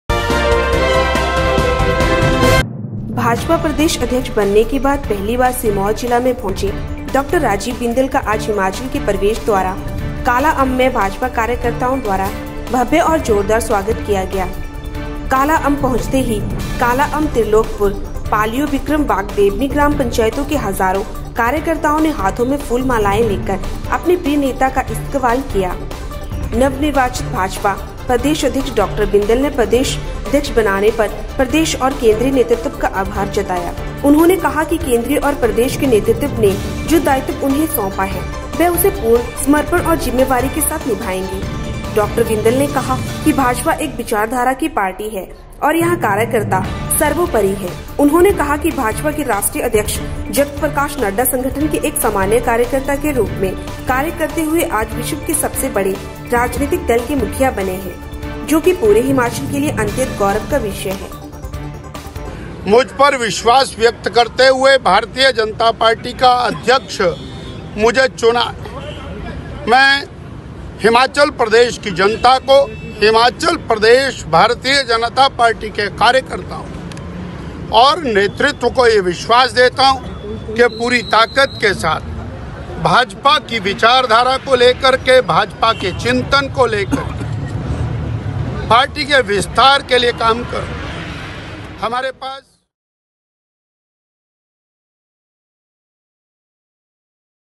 अध्यक्ष बनने के बाद पहली बार सिरमौर पहुंचे डा. बिंदल का जोरदार स्वागत
भाजपा प्रदेश अध्यक्ष बनने के बाद पहली बार सिरमौर जिला में पहुंचे डा. राजीव बिंदल का आज हिमाचल के प्रवेश द्वारा कालाआंब में भाजपा कार्यकर्ताओं द्वारा भव्य और जोरदार स्वागत किया गया।